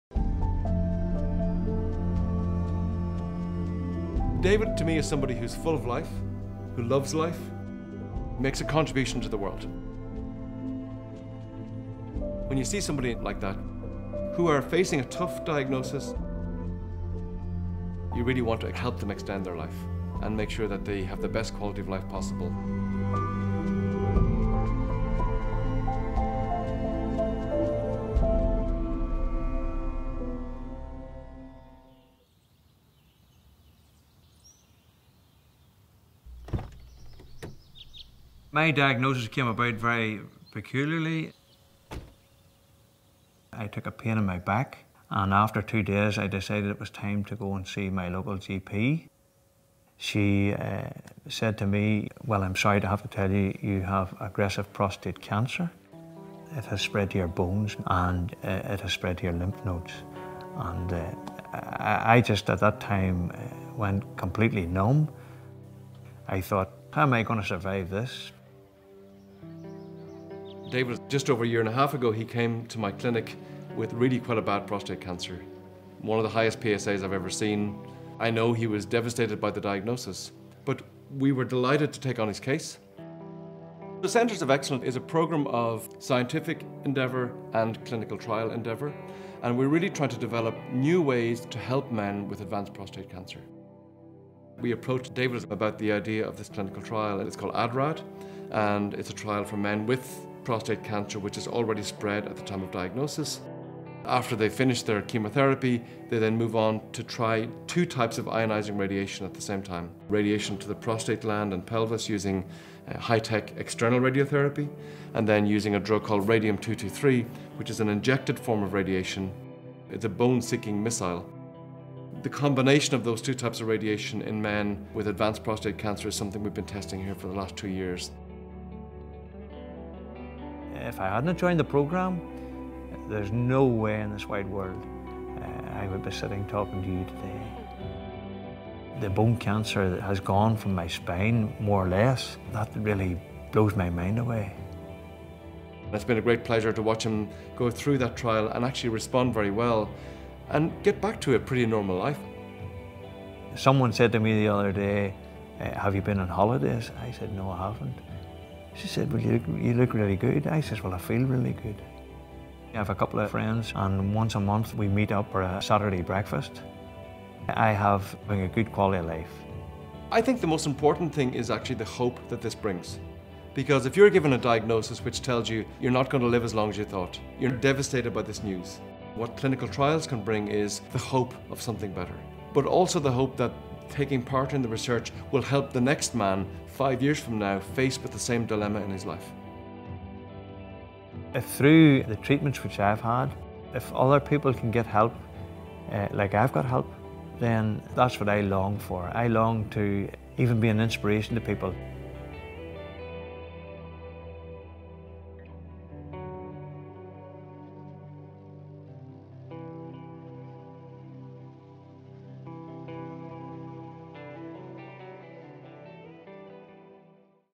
David's story
David, a grandfather of two, from Northern Ireland was asked to take part in a ground-breaking trial supported by the Movember Foundation which all but eradicated his prostate cancer.